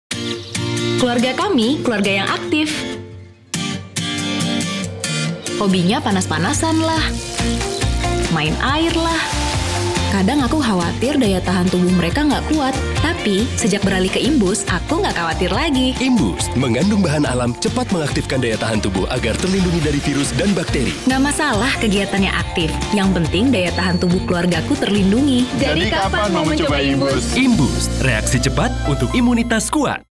Imboost - Keluarga Aktif (2017)
Our latest TV commercial developed for Imboost, portraying a healthy happy and very active family. What more would they need but a great supplement to get them through all the activities! Featuring Titi Kamal & Christian Sugiono.